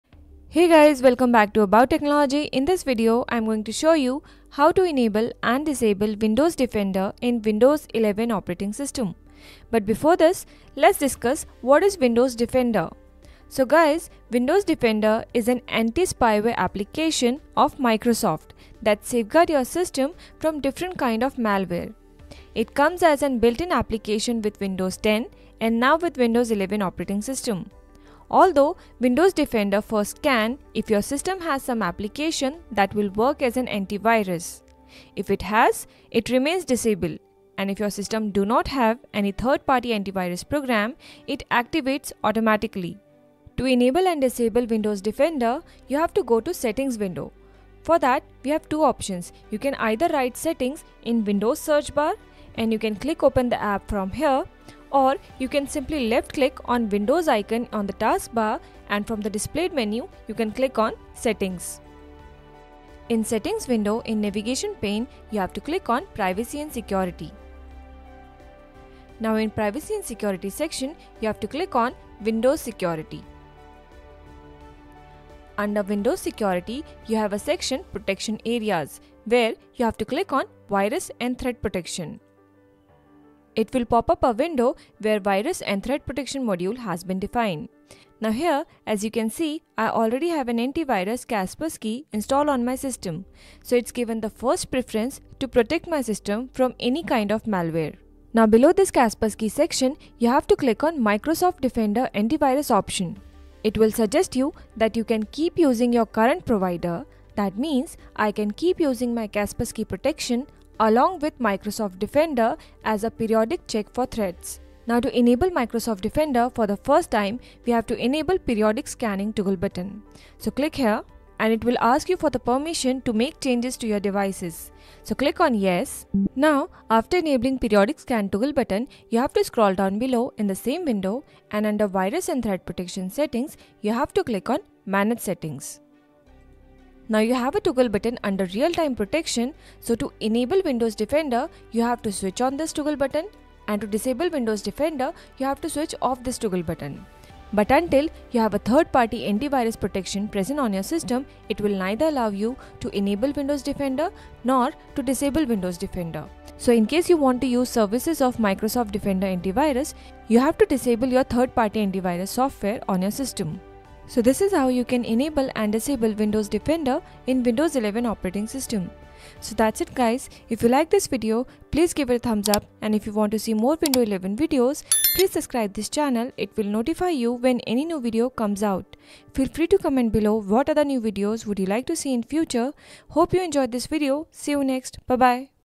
How to Enable and Disable Windows Defender in Windows 11
Windows Defender is a built-in application provided by Microsoft in the system with Windows operating system. It is responsible for protecting your system from different kinds of malware.

Although the Windows Defender priorities the other antivirus software if present on your system to serve as antivirus software. If no other antivirus system is present on your system, the Windows Defender gets activated automatically.